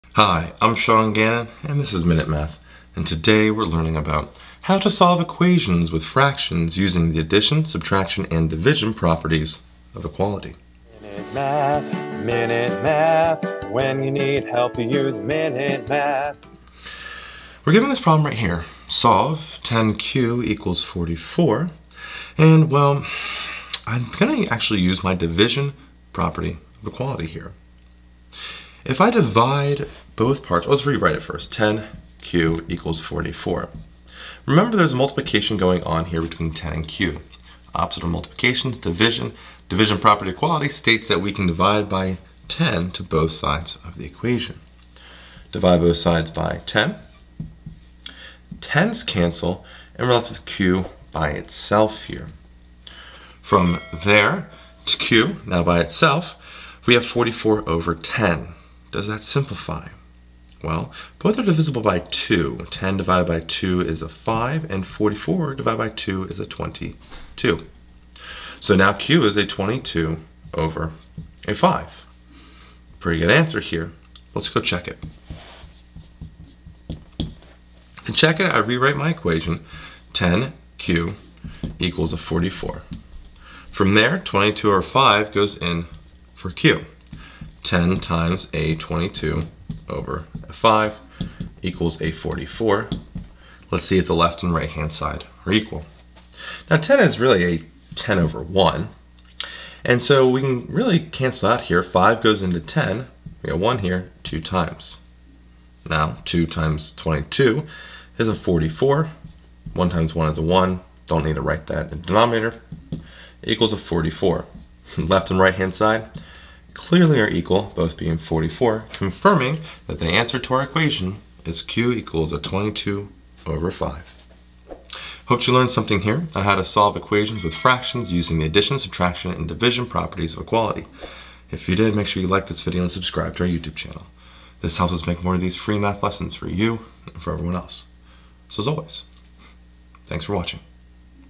How to Solve Equations with Fractions Using the Division Property of Equality | 10q=44 | Minute Math
In this video we learn how to Solve Equations with Fractions using the Addition, Subtraction, and Division Properties of Equality. We solve 10q=44.

The Addition, Subtraction and Division Properties of Equality.
For any numbers, a, b, and c,
if a=b, then a+c=b+c is the Addition Property of Equality
if a=b, then a-c=b-c is the Subtraction Property of Equality
if a=b, then a/c=b/c, c≠0 is the Division Property of Equality

Amazon Store Front: How we make our videos, Recommended Educational Resources, Etc.:

Marecek, L., Anthony-Smith, M., & Mathis, A. H. (2020). Use the Language of Algebra. In Prealgebra 2e. OpenStax.